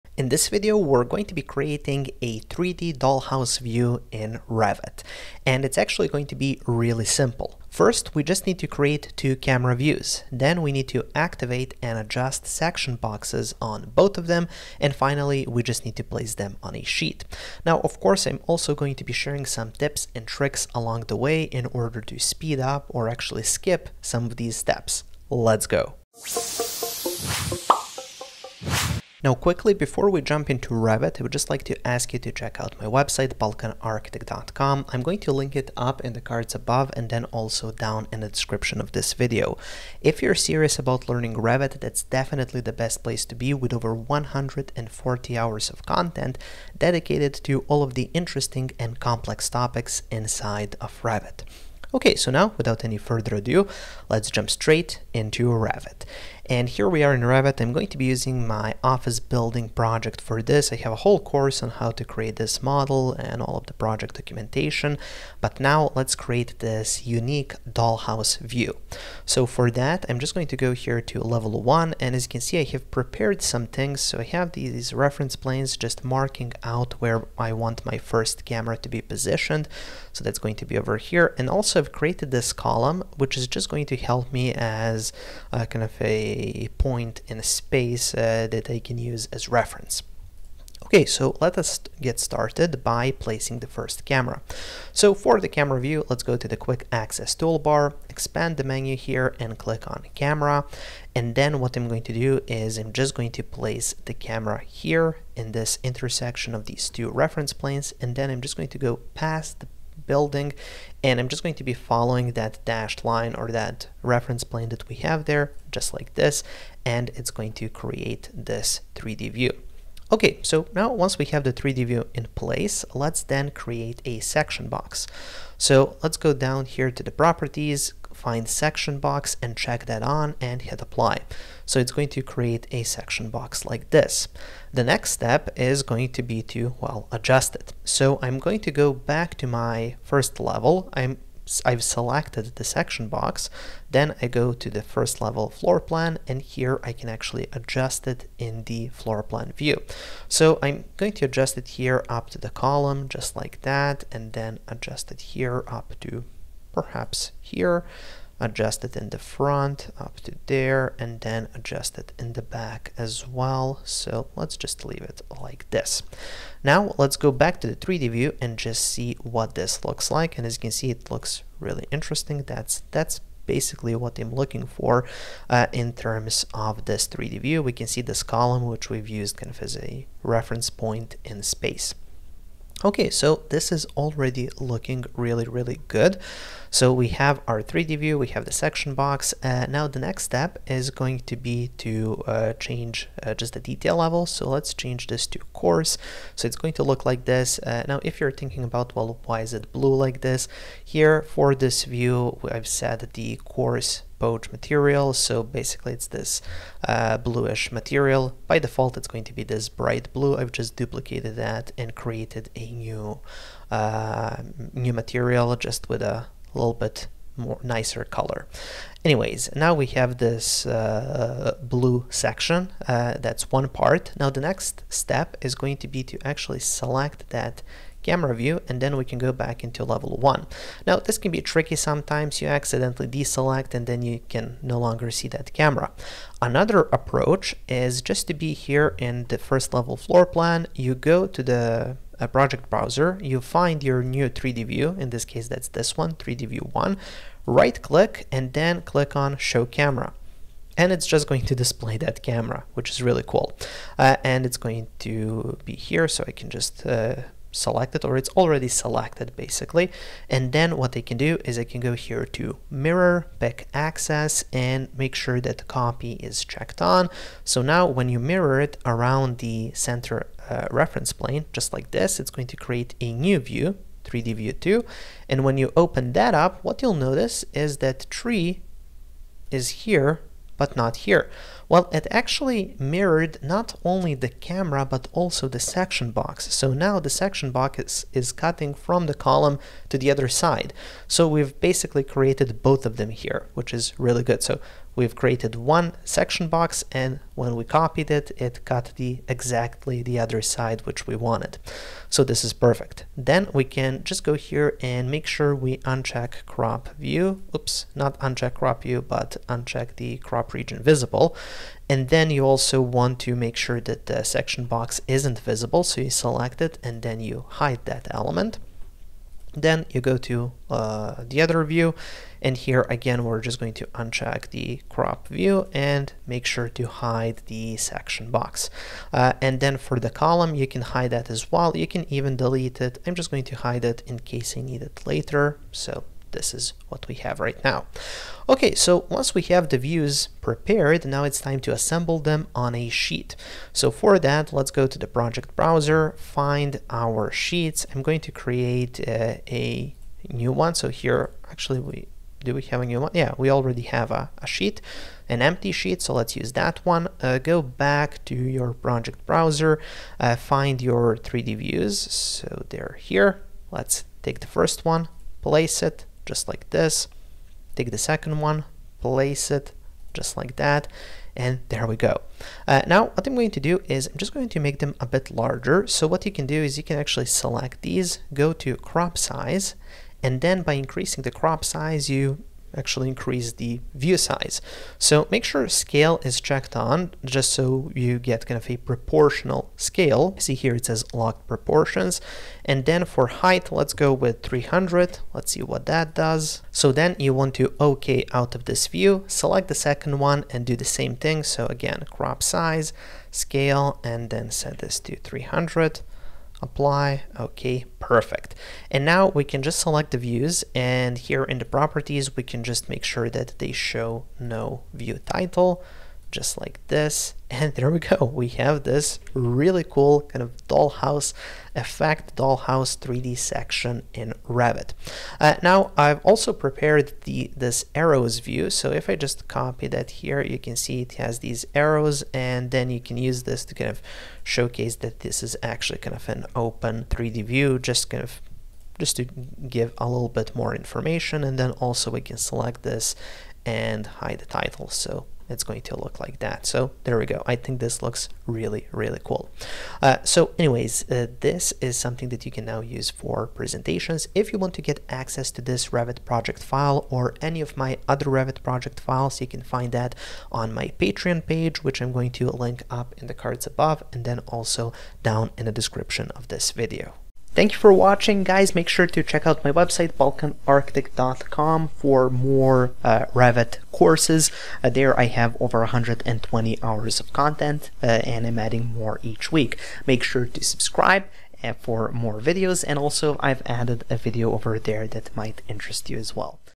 Barbie house 3D View in Revit Tutorial
In this Revit tutorial I will show you how to create a 3D Doll house view.
It's actually very simple, you just need to follow the steps!

Timestamps:

00:00 Introduction
01:33 Placing the first camera view
03:44 Placing second camera view
05:40 Placing views on a sheet

95YT23